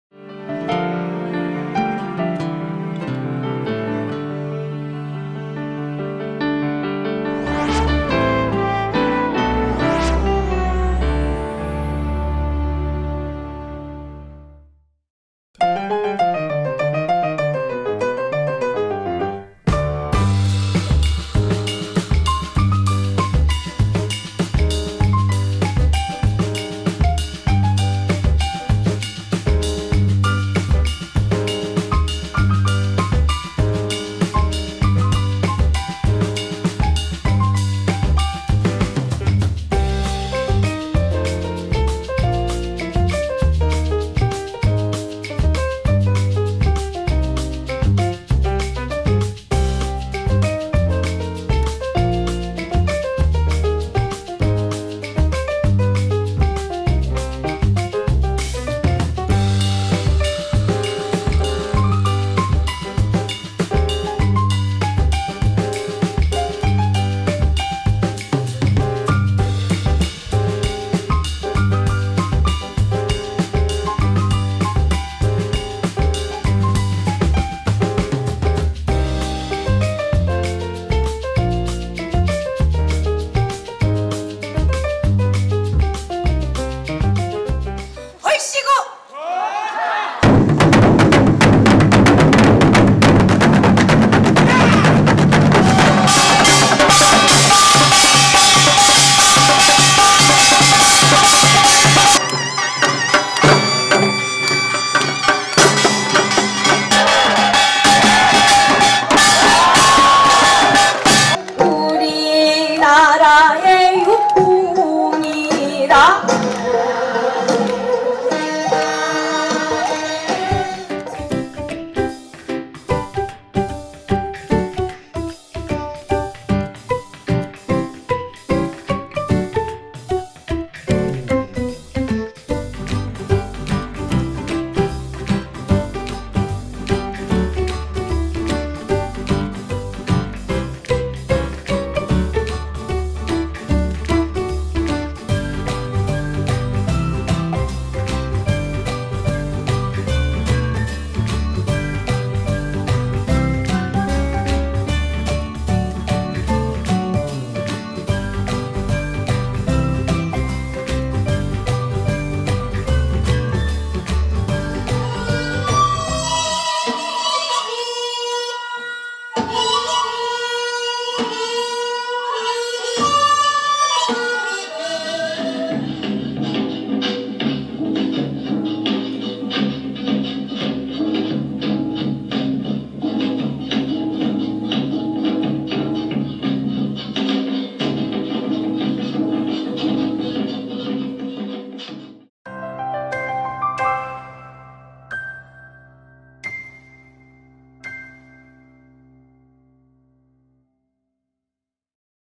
함께 나누고 즐기는 풍성한 한가위 대잔치 2014년9월6일 첫째 날
2014년 국립민속박물관에서 열리는 추석 명절 행사 내용과 안내 동영상 입니다